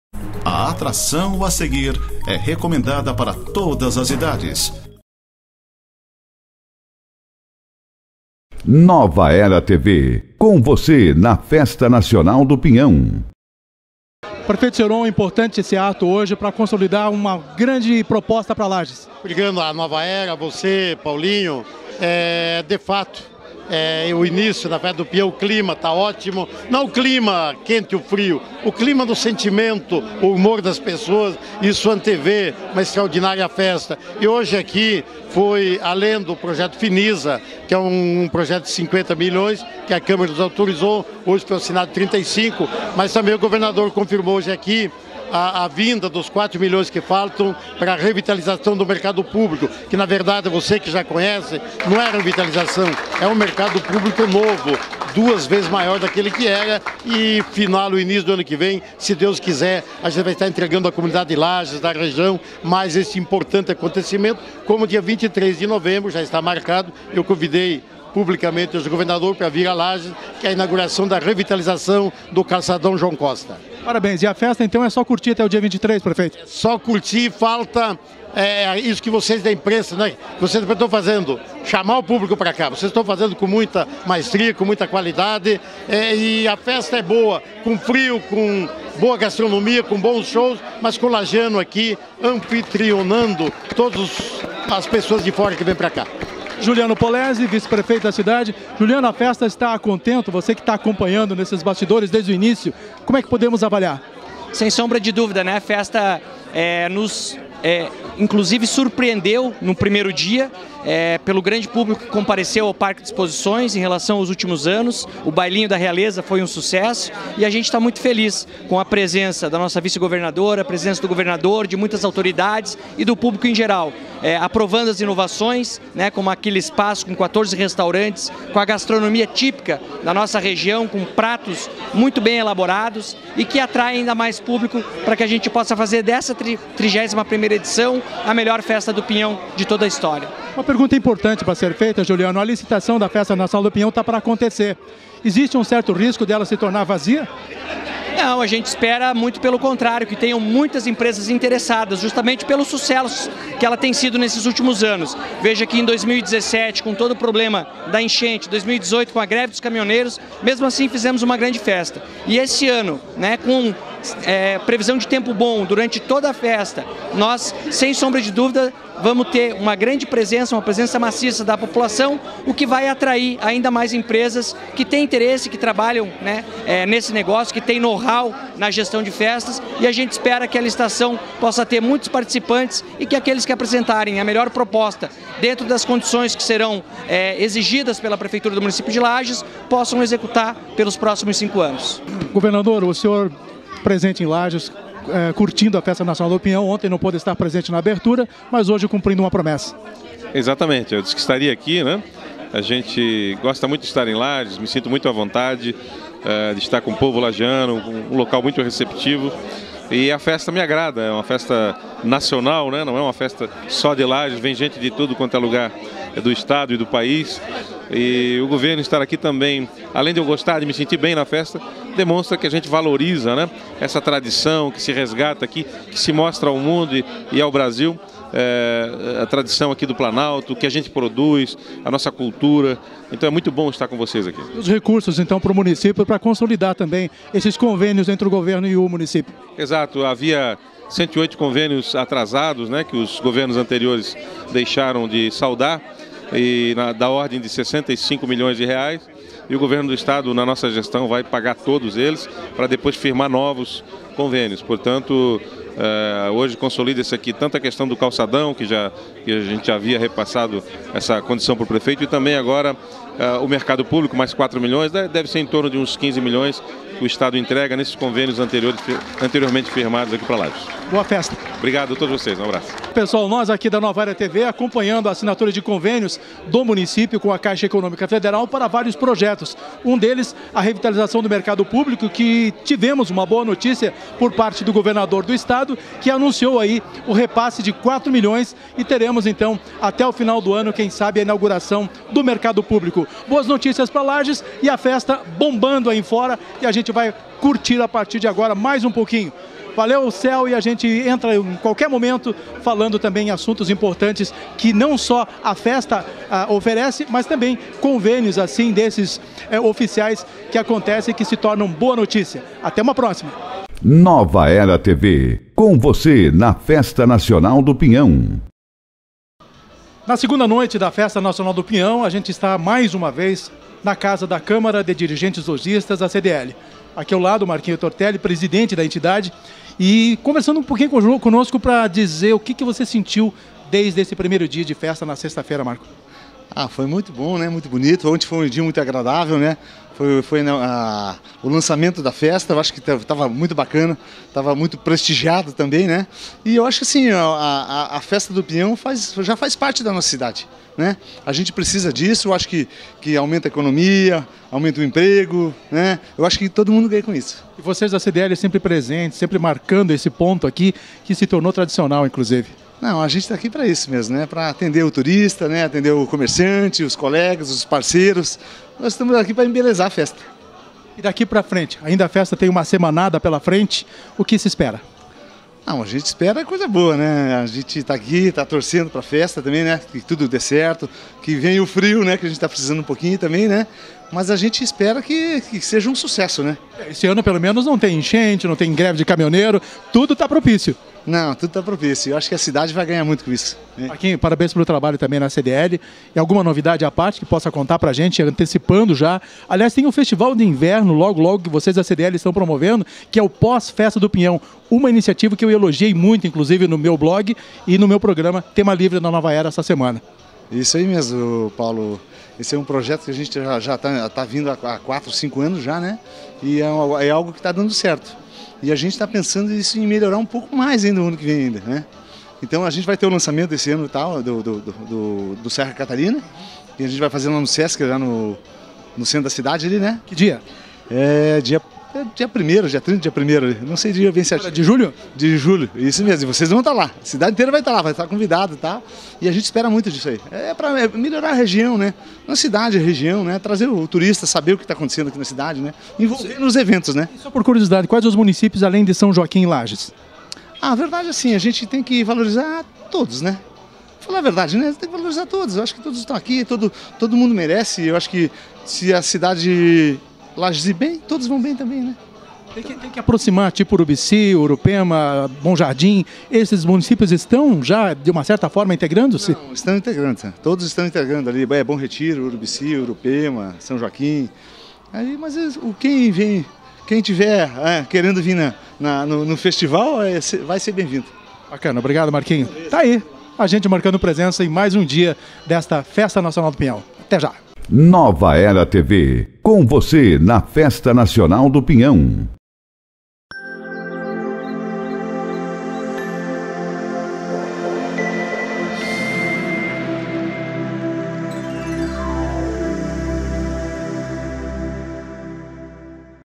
Festa do Pinhão Assinatura Financiamento de 35 milhões
Festa Nacional do Pinhão   Assinatura Financiamento de 35 milhões para Lages , com presença do Governador